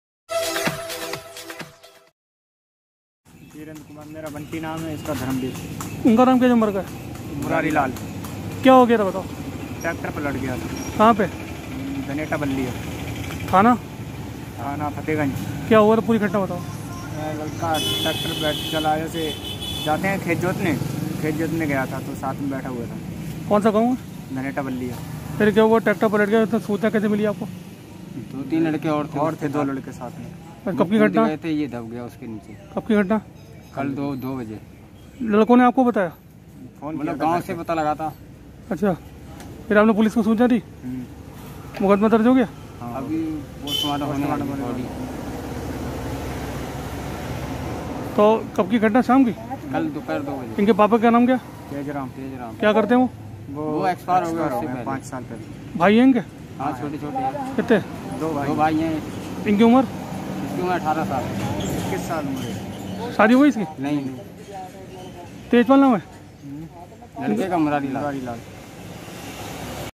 RGA न्यूज़:- ट्रैक्टर पलटने से युवक दवा मौके पर मौत
बरेली समाचार
बरेली ट्रैक्टर पलटने से युवक की मौत थाना फतेहगंज पश्चिमी के गांव में रहने वाले तेज राम उम्र 21 वर्ष ट्रैक्टर से खेत में जुताई करने के लिए जा रहा था तभी रास्ते में ट्रैक्टर पलटने से नीचे दब गया जिससे उसकी मौत हो गई मौत की खबर मिलते ही परिवार में कोहराम मच गया पुलिस ने शब को पंचनामा भरकर पोस्टमार्टम के लिए बरेली भेजा